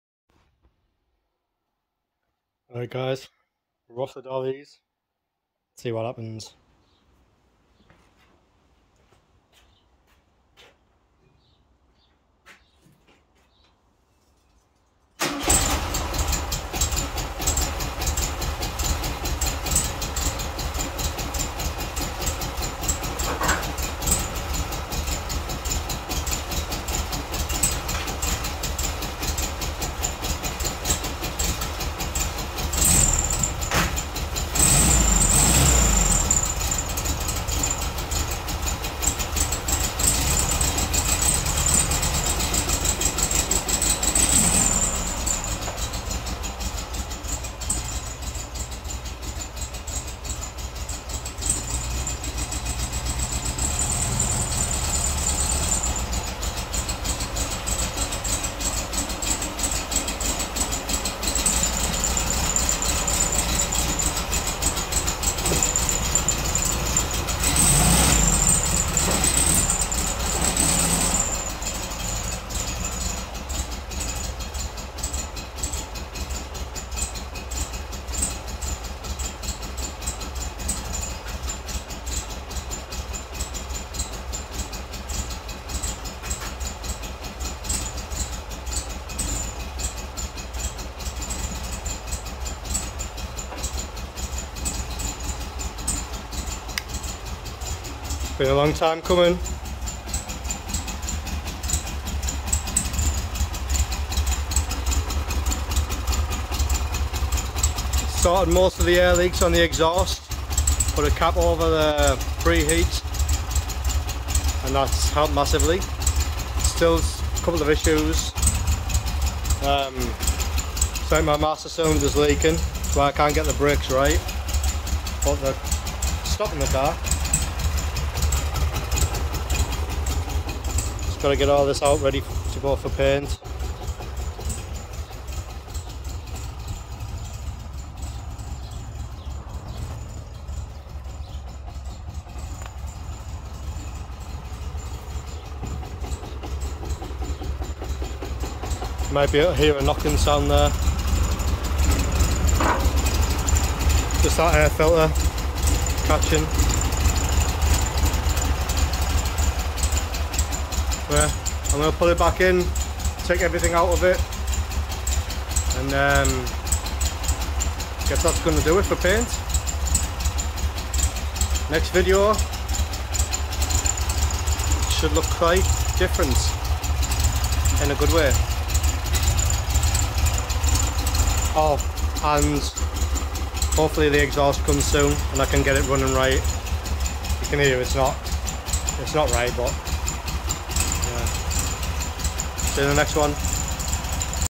1973 GT Beetle finally driving again and ready to be painted.
My 1973 GT is finally driving again and all ready to go and get painted.